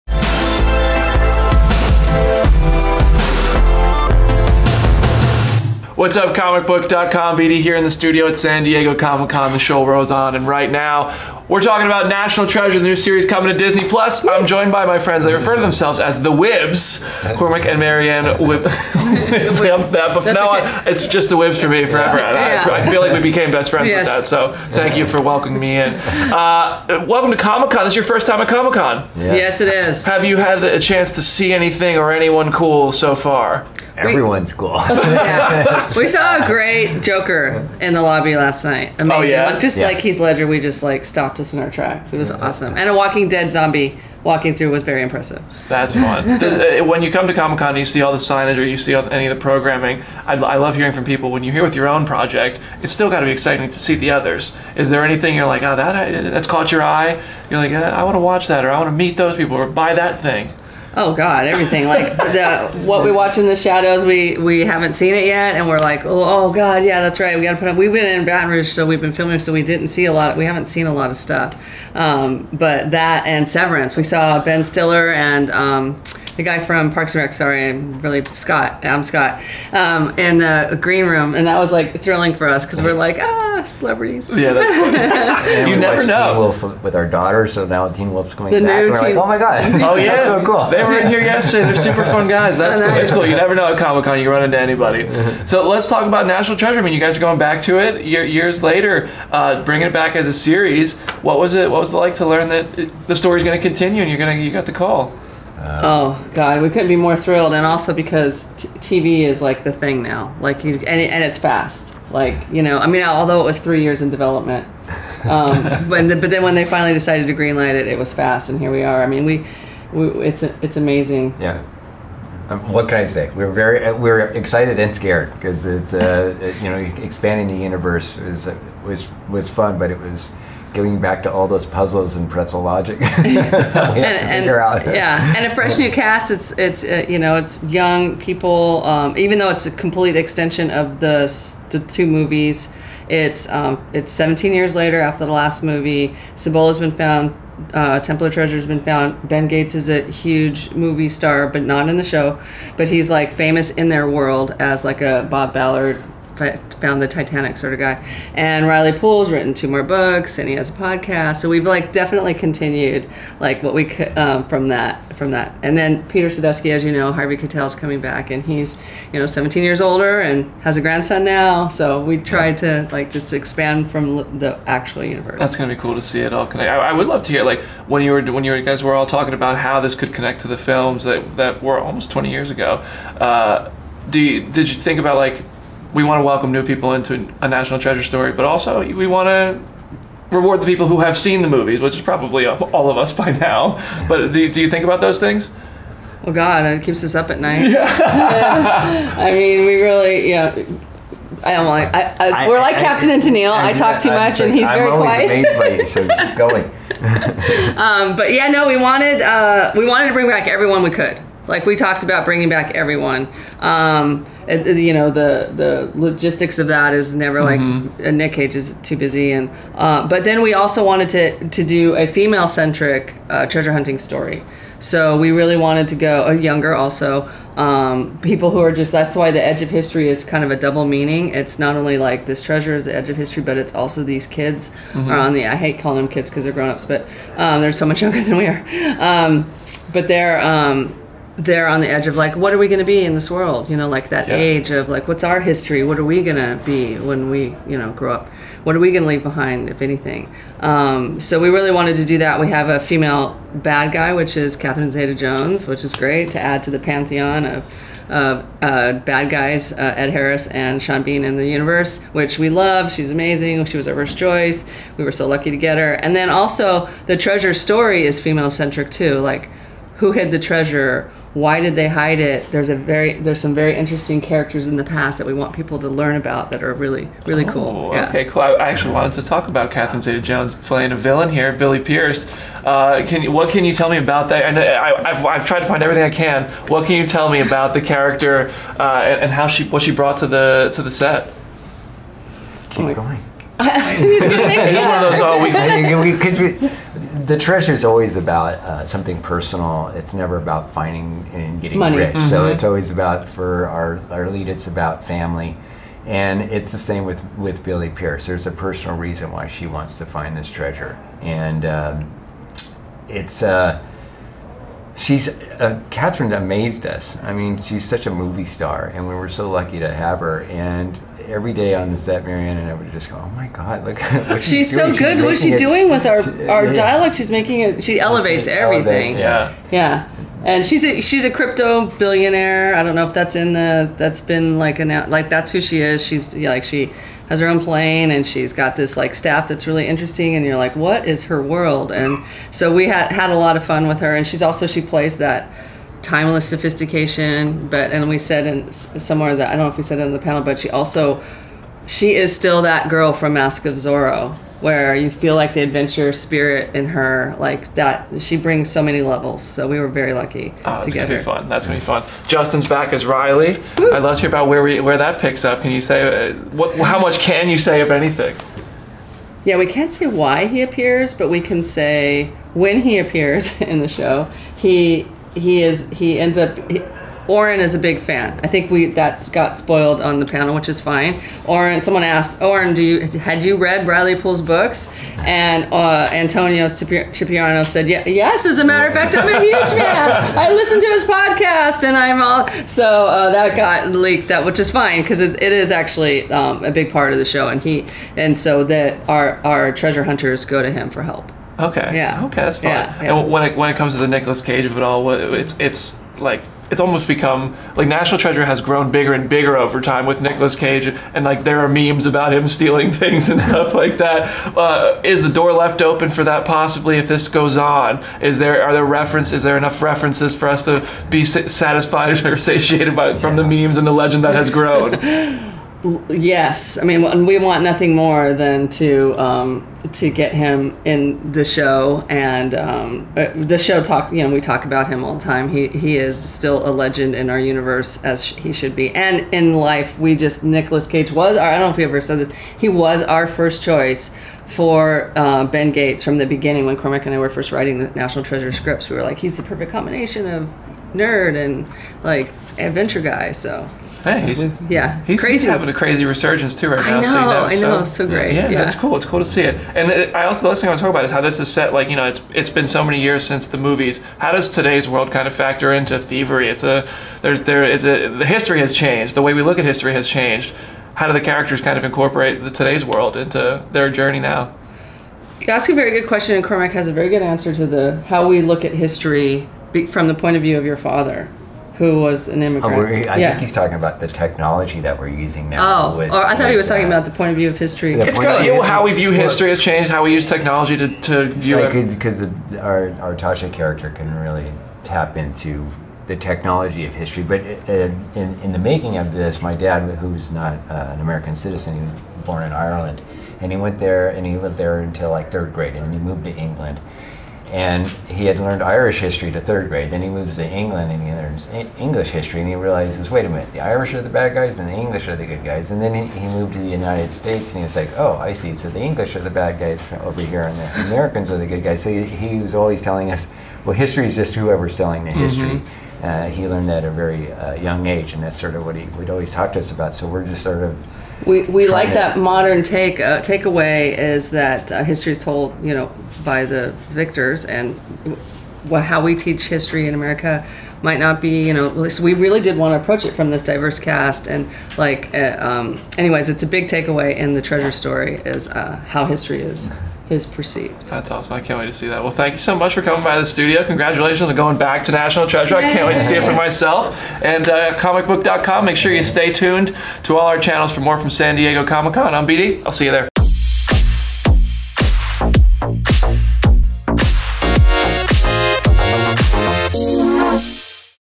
How National Treasure: Edge of History Will Be Different From the Movies | 2022 San Diego Comic-Con
Sitting down with ComicBook's Brandon Davis at San Diego Comic-Con 2022 National Treasure: Edge of History's Producer and Writer Cormac Wibberley and Marianne Wibberley discuss Catherine Zeta-Jones' villain, how the TV series will be different than the films + MORE!